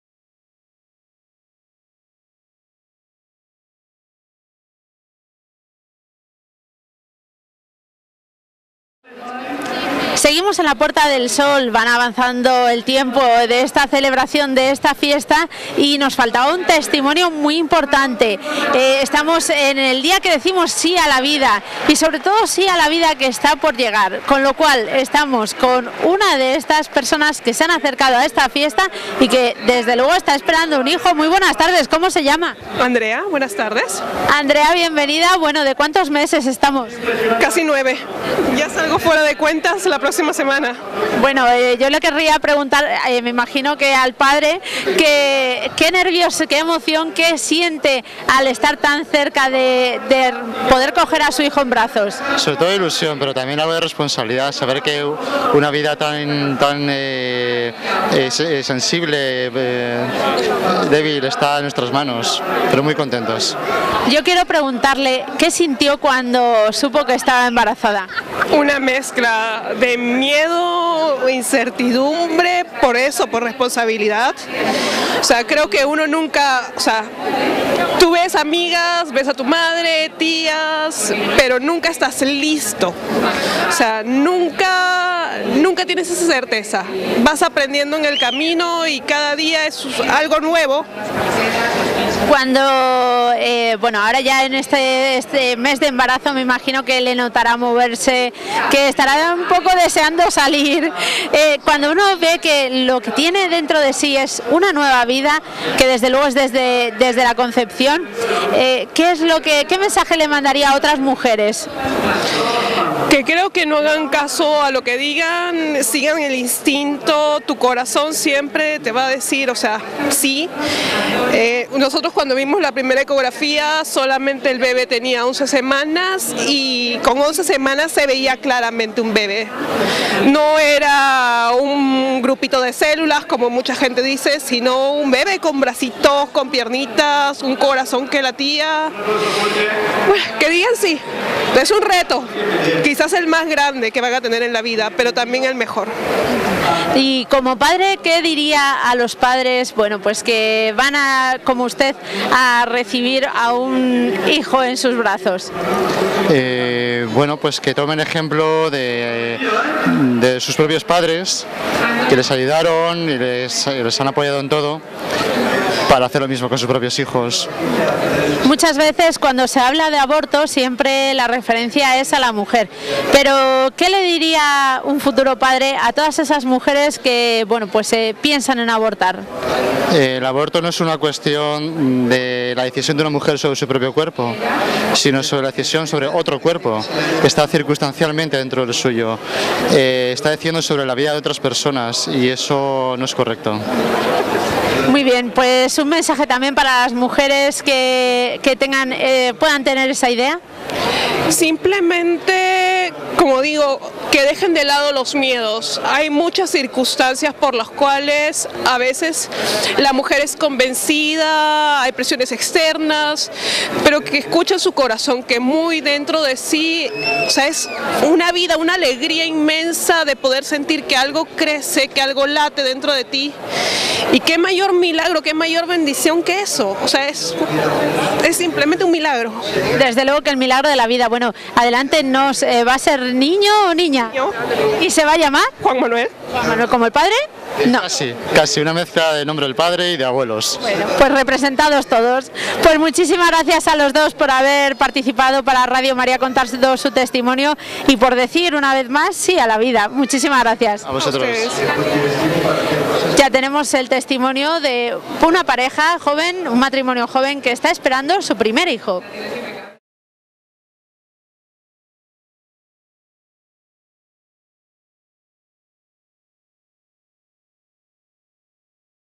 Joven matrimonio esperando hijo.mpg
Testimonio de un matrimonio a la espera del nacimiento de su hijo. Jornada Provida en la Puerta del Sol de Madrid. Entrevista Mónica Martínez para Radio Maria.